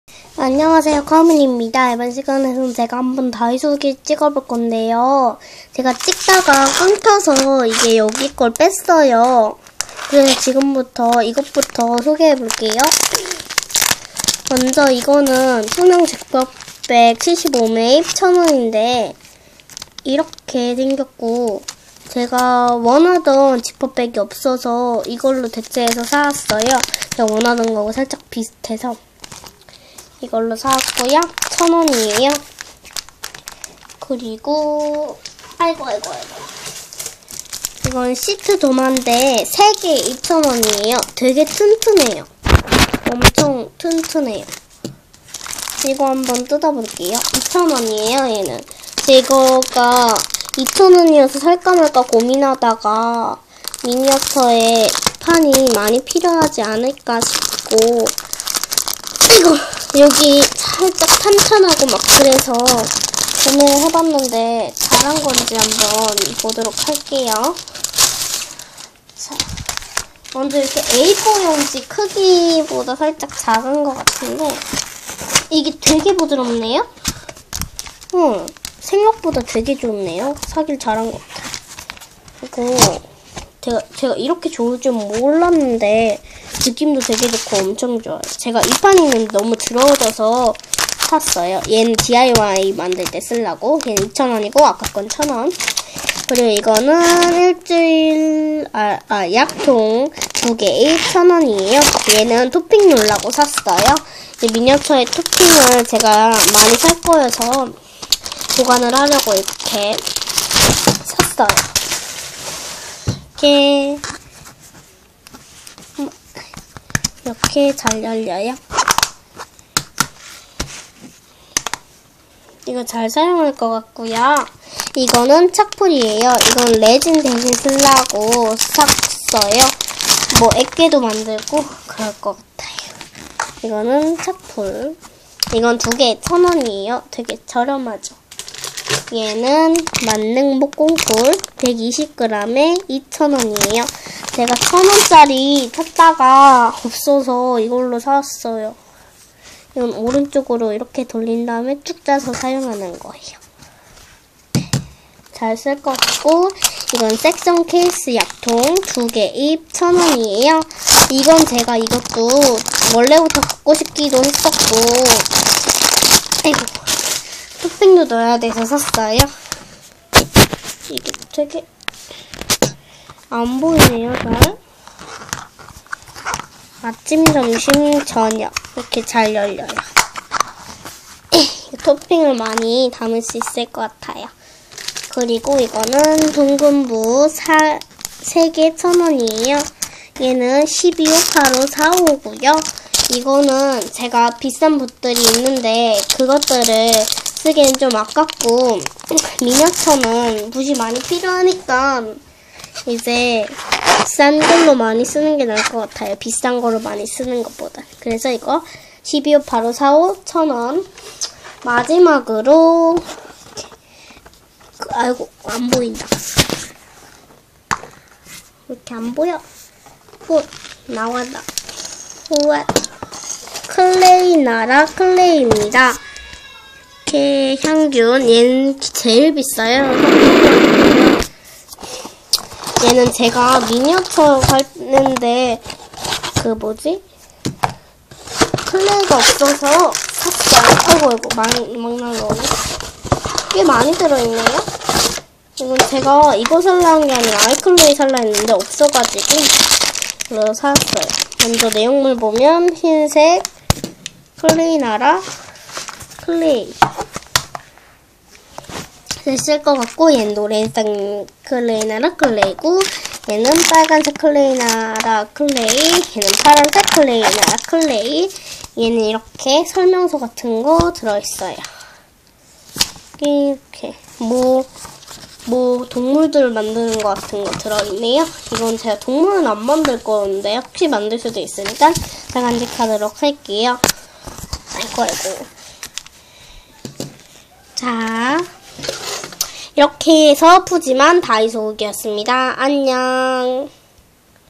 푸짐한 다이소 후기~^^
오랜만에 다이소!푸짐하게 12000원치 사왔다능~^^